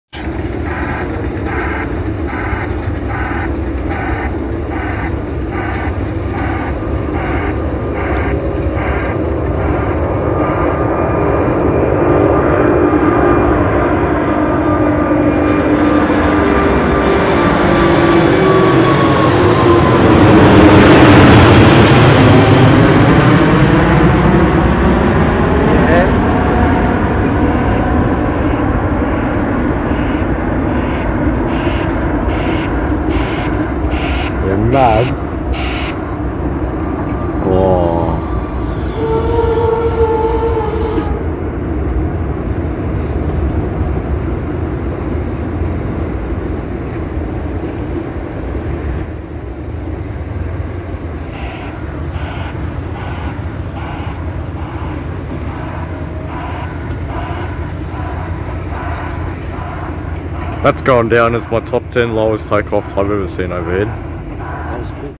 INCREADIBLE SUNSET A380 QANTAS HEAD ON TAKEOFF LOW & LOUD!
What a heavy beast going to Singapore via London on a beautiful Aussie winter sunset afternoon :).